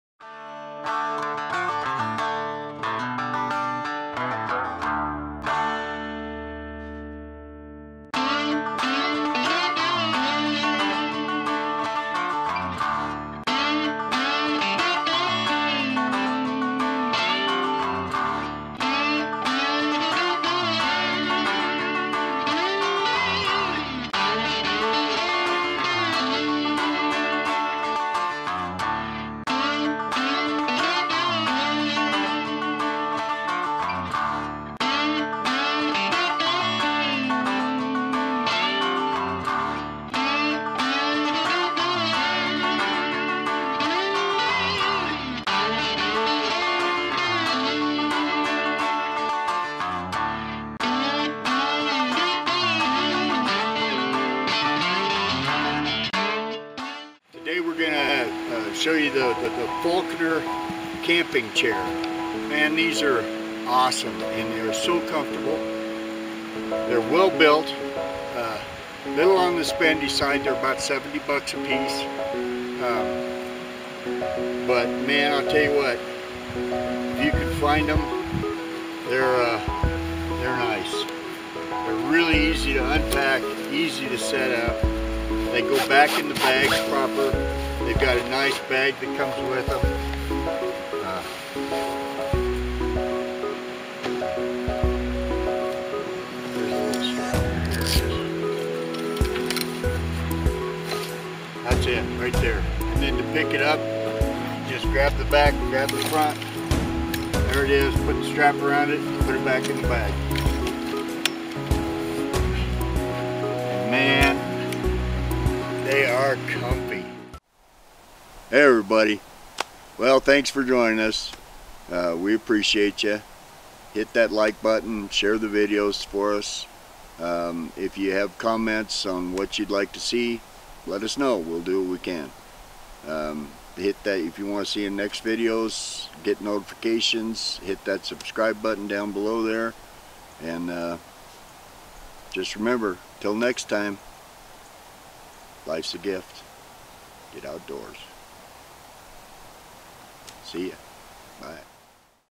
REVIEW OF THE FAULKNER CAMP CHAIR --Best camping chair-- Over sized camping chair.
REVIEW OF THE FAULKNER CAMP CHAIR --Best camping chair-- Over sized camping chair.  A must have camping chair for 2020.  I'm reviewing and setting up of the Faulkner Camp Chair.  One of the best camping chairs for big guys, very well made.

Faulkner camp chair

Action Camera
Mini Tri Pod
Memory card
Tri pod

the Faulkner folding camp chair.  a review of the Best folding camp chair I have ever owned.